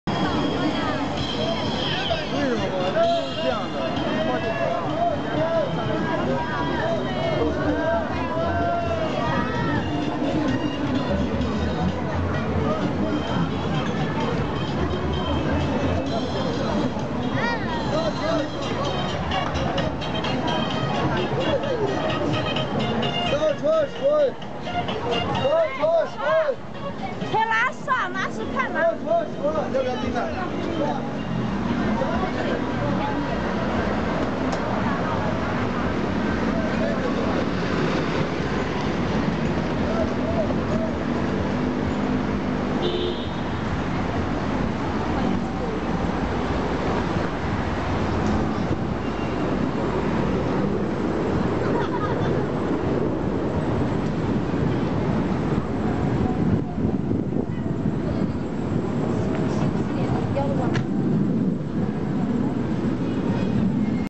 Main Drag Near my Apartment
Here is a video of the street life near my apartment